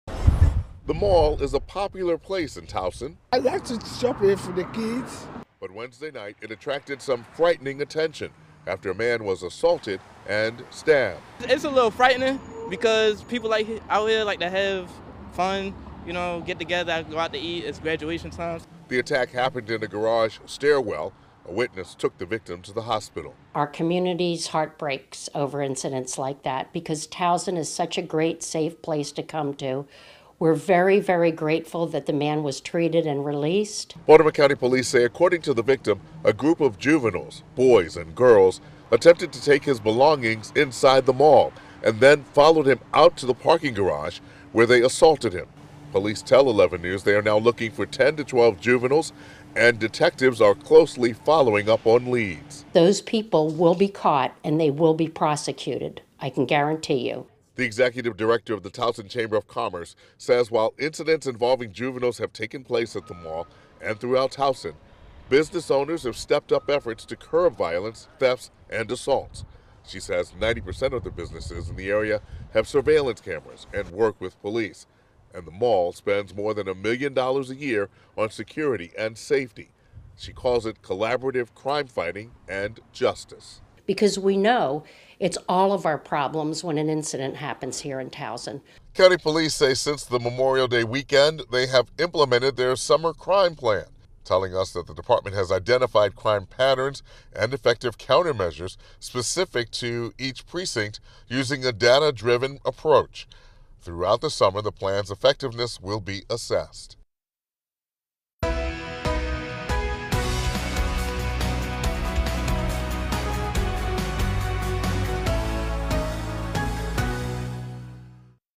Mall parking garage stabbing victim released from hospital
A Towson Town Center customer was assaulted and stabbed while leaving the mall, police said. Baltimore County police said officers were called around 8:30 p.m. Wednesday for a fight in progress in the 800 block of Dulaney Valley Road. Police said witnesses told officers that a group of about 10 to 12 boys and girls assaulted and stabbed an adult victim. A witness took the victim to a hospital, where he was treated for non-life-threatening injuries and released, police said.